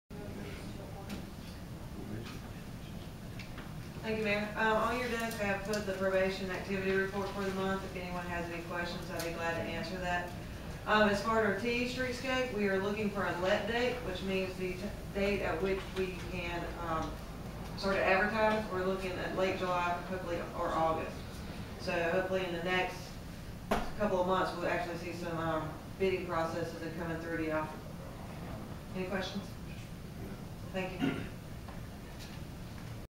5. Dept. Reports. a. City Clerk b. Probation @ RCC 2012-06-04
5.a. City Clerk -- Rachel Tate
5.b. Probation -- Jessica Freeman
Work Session, Remerton City Council (RCC)
Remerton, Lowndes County, Georgia, 4 June 2012.